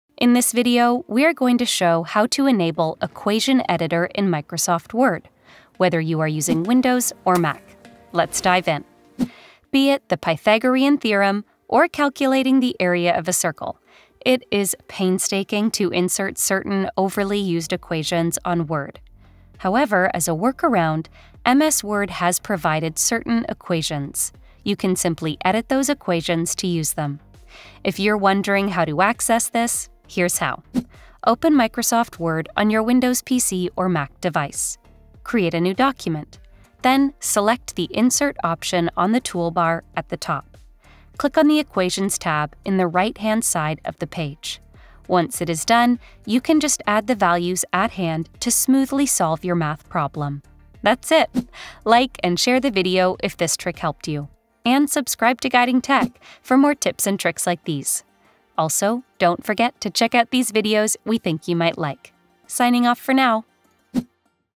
How to Enable Equation Editor in Microsoft Word | Full Guide | Guiding Tech
Be it the Pythagoras theorem or calculating the area of a circle; it is painstaking to insert certain overly used equations on Word. However, as a workaround, MS Word has provided certain equations.

You can simply edit those equations to use them. If you’re wondering how to access this, here's how.

00:00 Intro
00:09 About the feature
00:32 Accessing The Feature
00:52 Outro